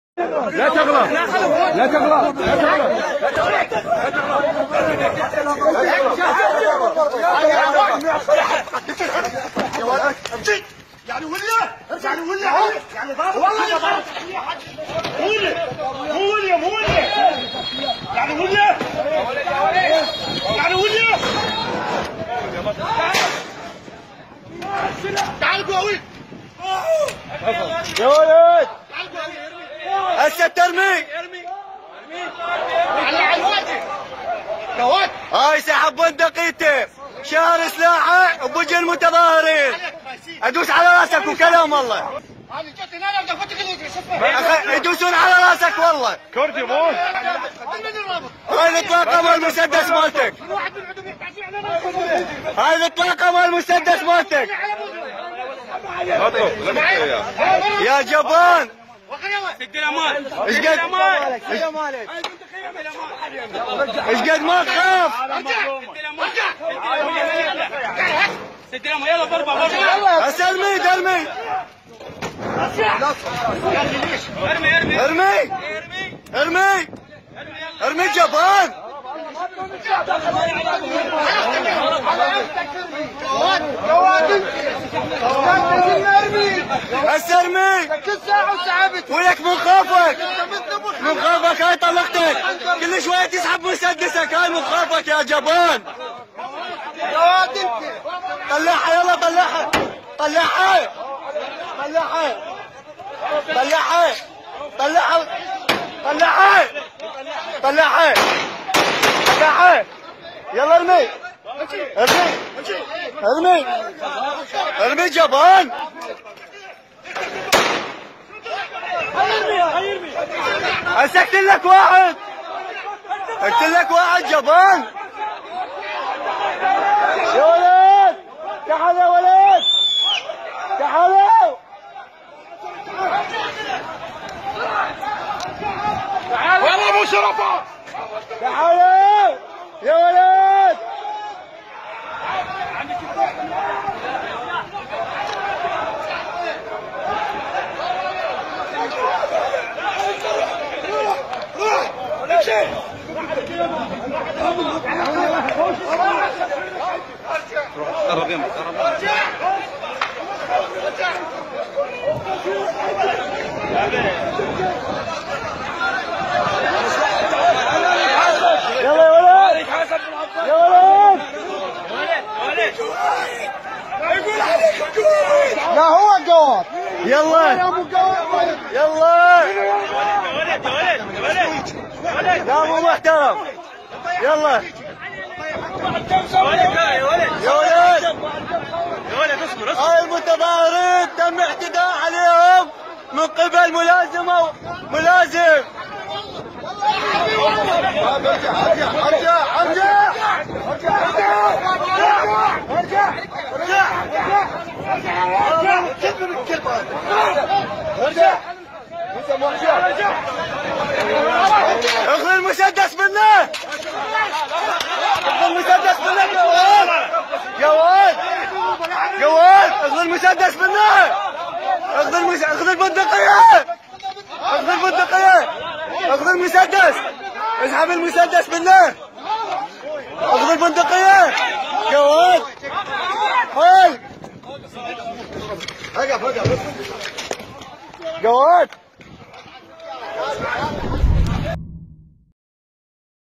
شاهد.. متظاهرون عراقيون يعتدون على ضابط شرطة بعد مشادة كلامية
أكد بيان لقيادة العمليات المشتركة في العراق أن القوات الأمنية قادرة على الدفاع عن أمن البلاد ضد أي اعتداءات خارجية
وطالب البيان تركيا بحل مشكلاتها بعيدا عن الأراضي العراقية, يأتي ذلك بينما أعلنت تركيا تمسكها بعملياتها العسكرية ضد المقاتلين الأكراد في شمال العراق